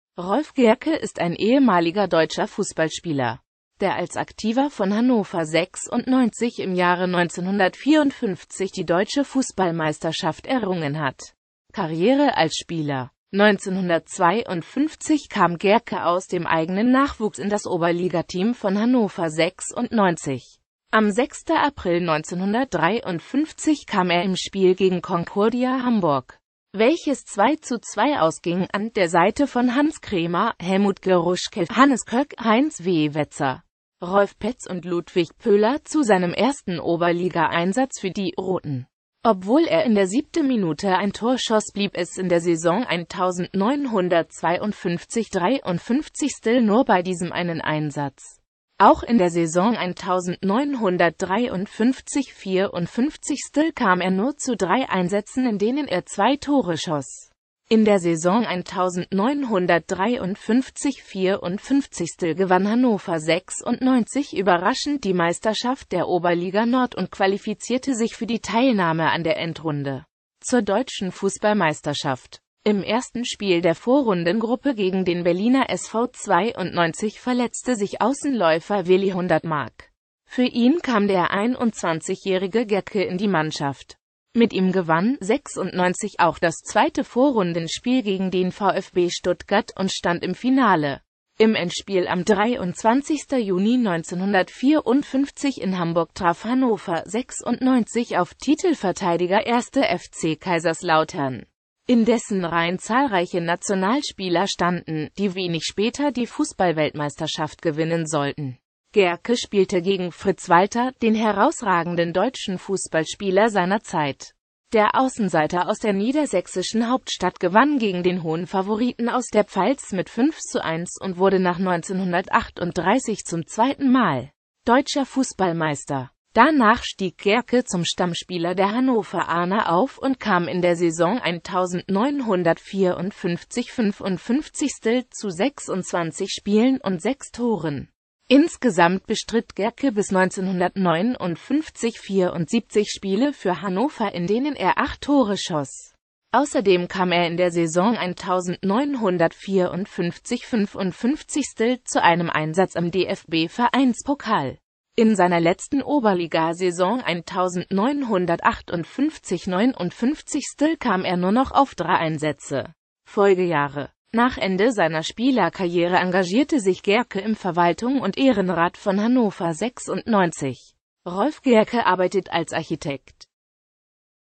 Rolf Gehrcke
Vielen Dank für Ihre Unterstützung:

Rolf Gehrcke (* 8.November 1932) ist ein ehemaliger deutscher Fußballspieler, der als Aktiver von Hannover 96 im Jahre 1954 die Deutsche Fußballmeisterschaft errungen hat.